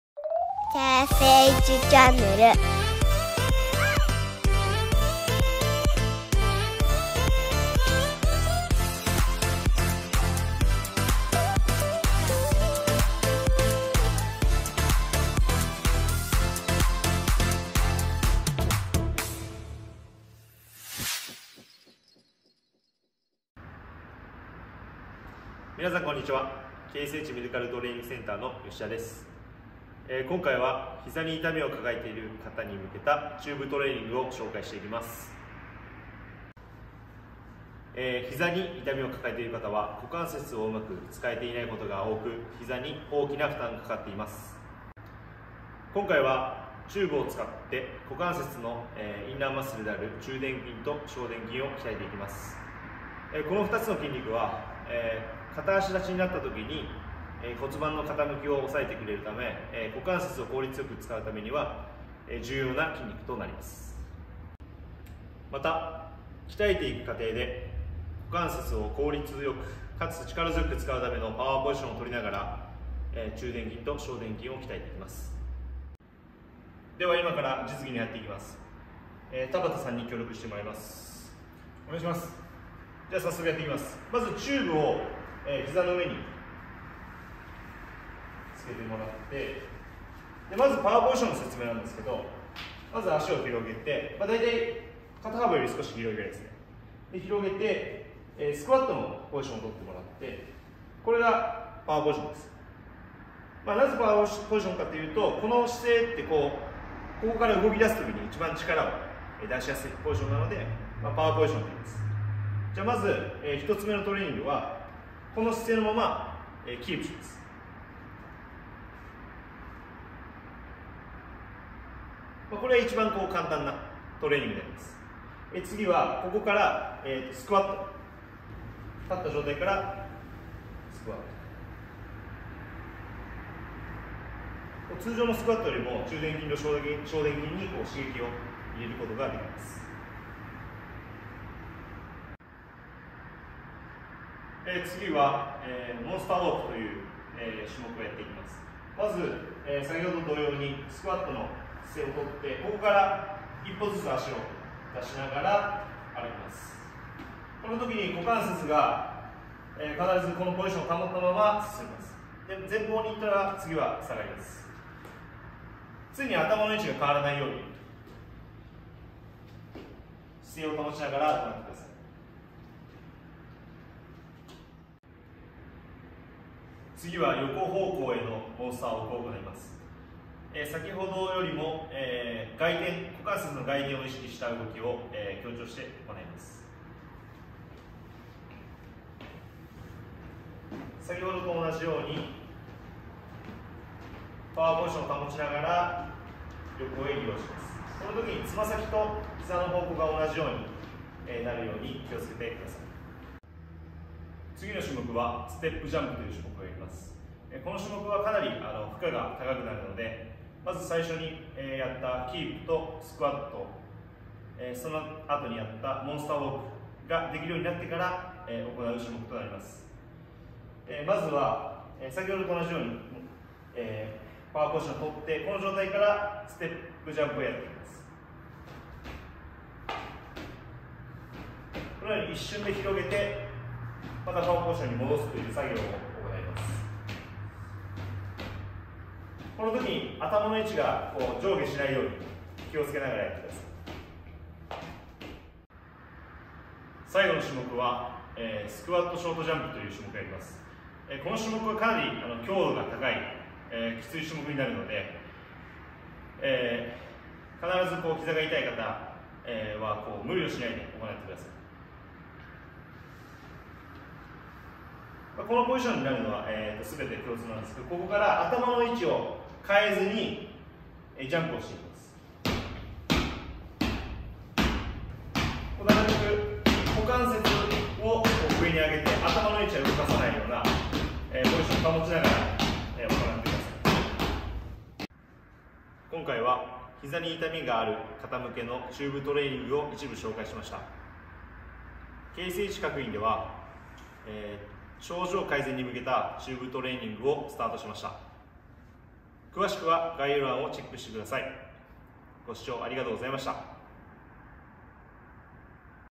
膝の痛みがある方必見！チューブトレーニングで膝の痛み改善！
チューブトレーニングで痛みの改善を！

身体の不調でお困りの方は是非ホームページをチェックしてください！！
※無料相談窓口やってます○

〈KSHグループ〉
愛知県岡崎市、三重県桑名市を中心に整骨院、トレーニングジム、デイサービスを展開。